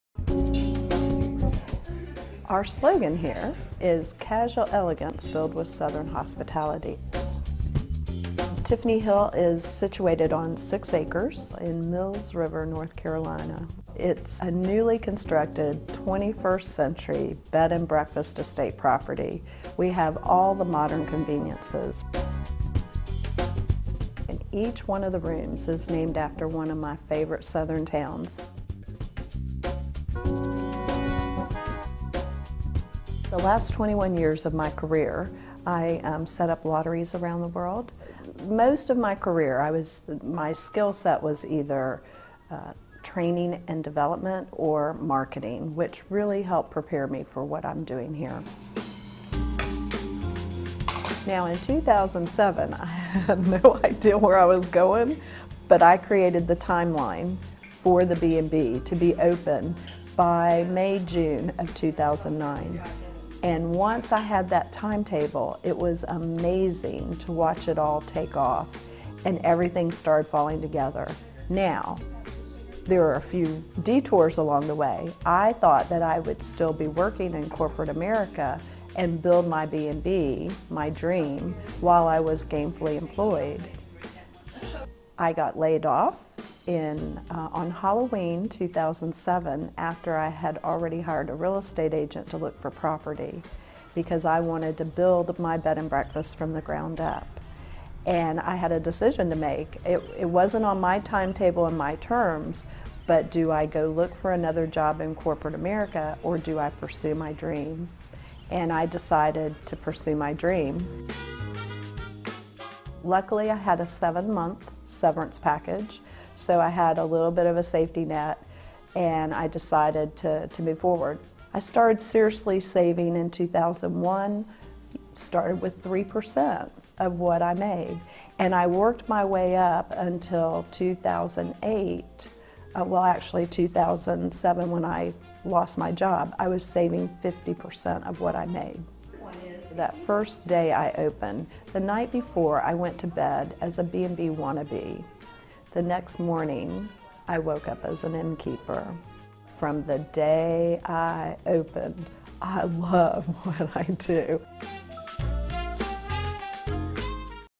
Here's How An Innkeeper Launched Her Dream Business | Money | TIME
Selena Einwechter, innkeeper at North Carolina's Bed & Breakfast on Tiffany Hill, explains how she launched her dream business. com/money/3967532/innkeeper-tiffany-hill-dream-job-road-to-wealth/